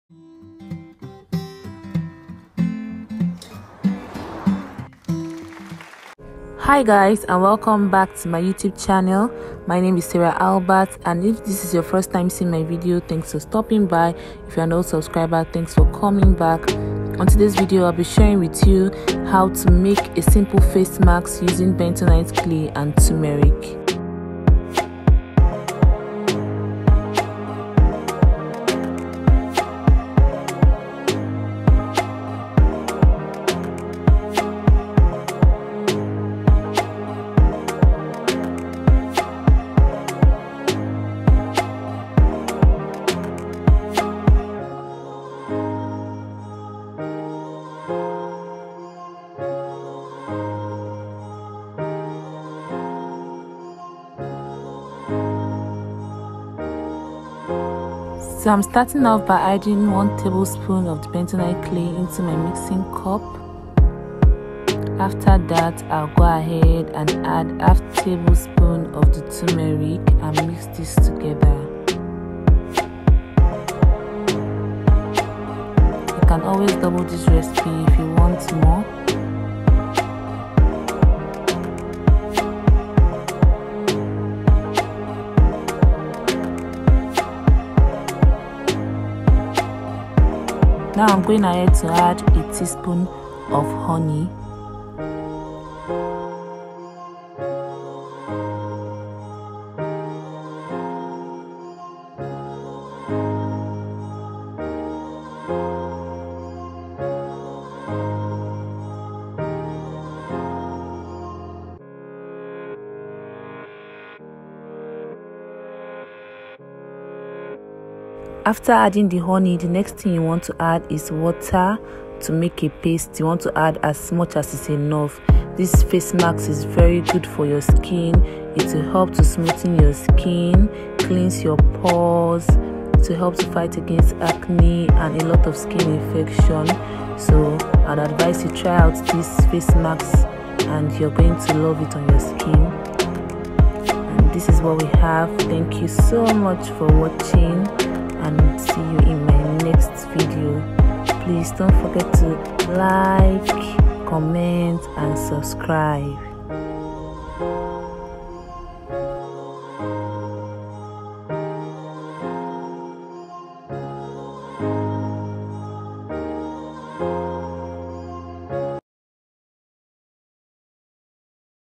DIY Clay mask with Tumeric| No more acne| Homemade Facemask
Hey loves, are you struggling with acne and blackheads? Here is a simple face mask you can make at home to take care of your skin.